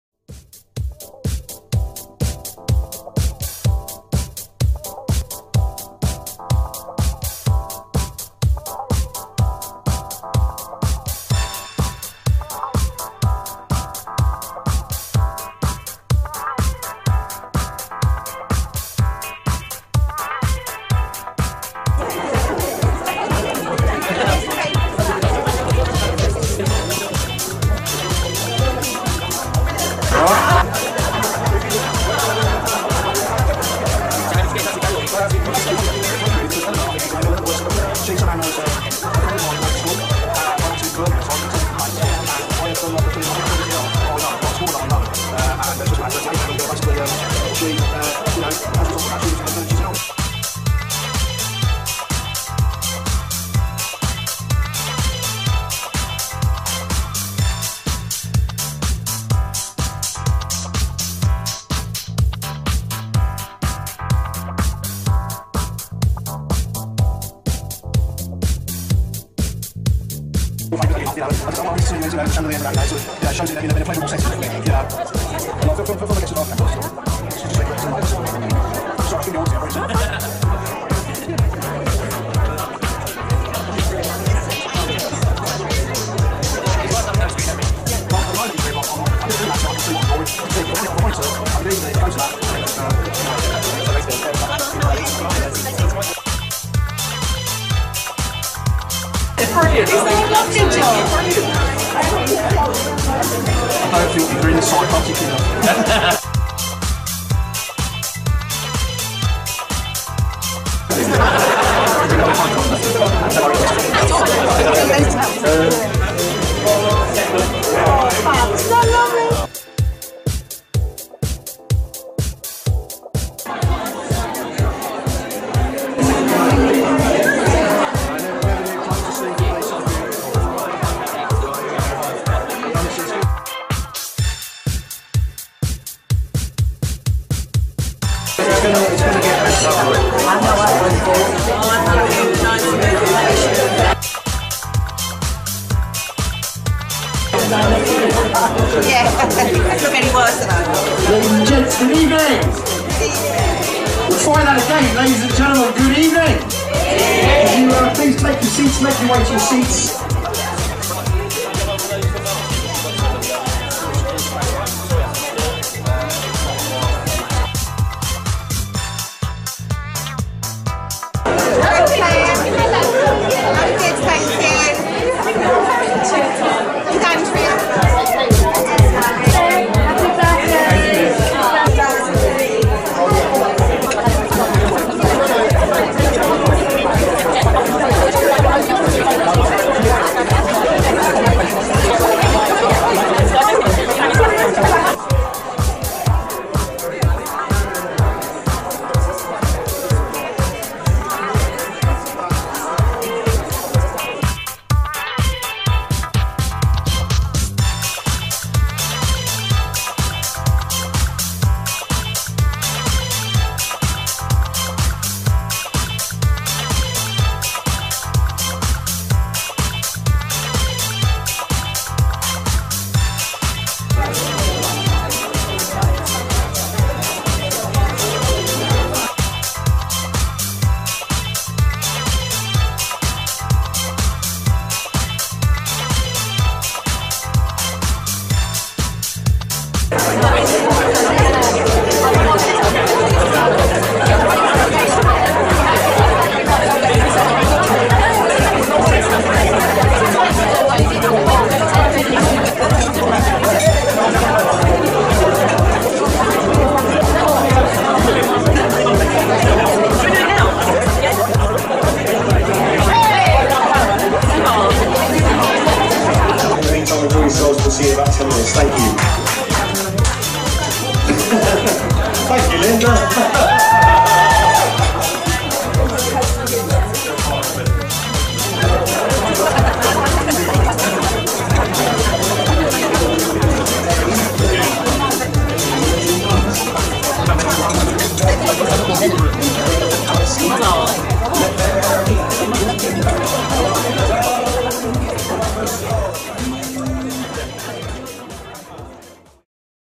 Rich The Caricaturist - iPad Experience // Live Digital Caricatures
Watch caricature come to live in real time on a display TV with Rich The Caricaturist. Hilarious pictures of your guests drawn on an iPad in a few minutes to share on social media with friends after the event.